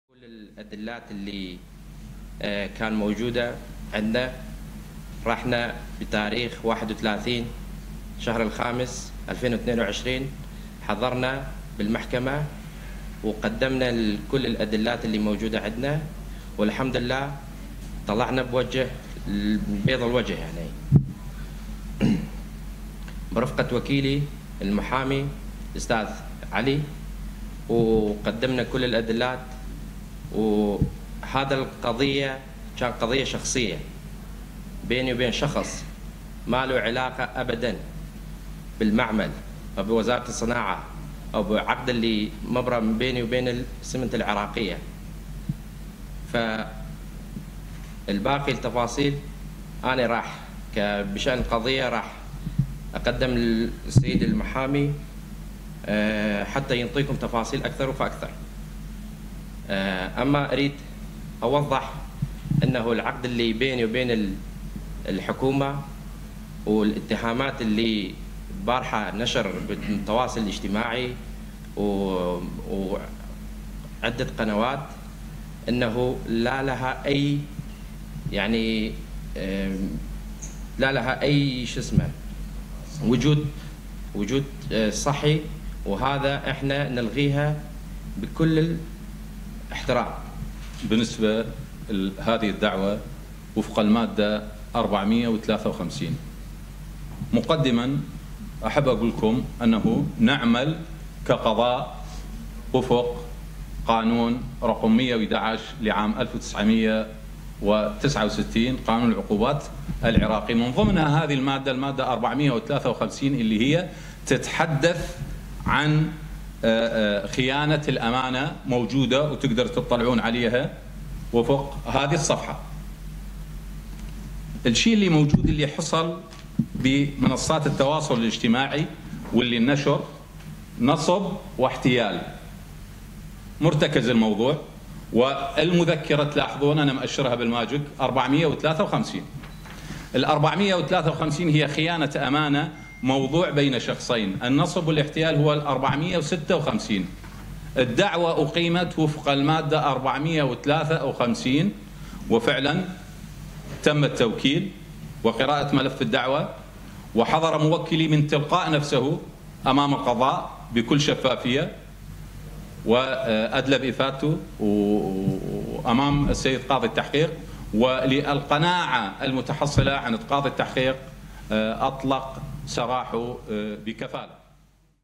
مستثمر معمل اسمنت بادوش الجديد يتحدث عن مذكرة القبض الصادرة بحقه ويؤكد اثبات براءته
مستثمر معمل اسمنت بادوش الجديد يتحدث عن مذكرة القبض الصادرة بحقه ويؤكد مثوله أمام القضاء واثبات براءته